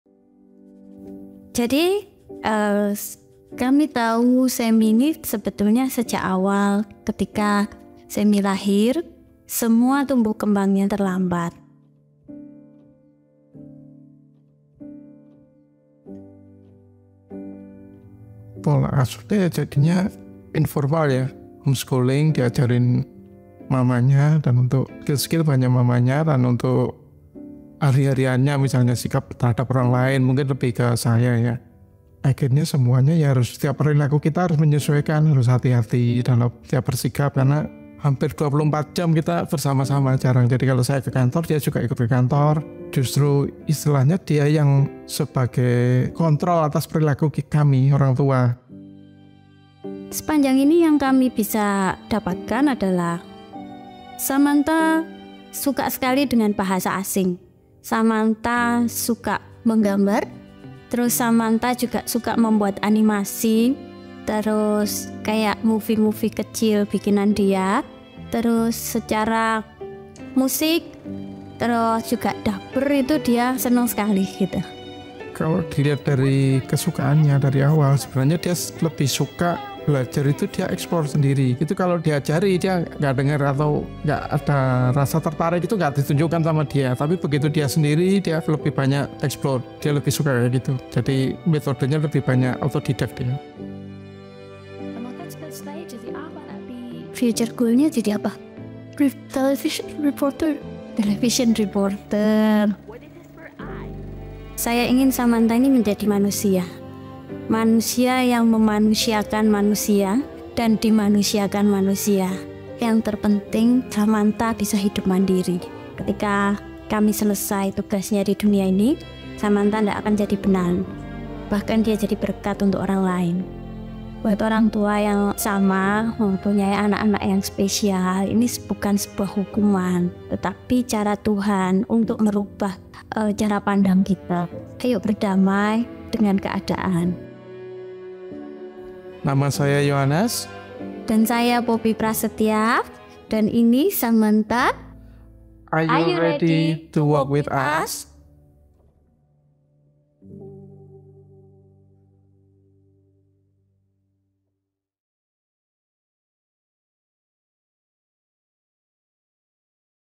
Episode 2: Samantha, The Self-Learner (ID Sub) | A WalkForAutism Documentary Series
"Saya ingin Samantha ini menjadi manusia, manusia yang memanusiakan manusia dan dimanusiakan manusia." - Poeppy Prasetya

Saksikan kisah inspiratif dari kedua orang tua Samantha dalam persiapannya agar ketika tugas mereka sudah selesai di dunia ini, Samantha tidak menjadi benalu namun bisa hidup mandiri bahkan menjadi berkat.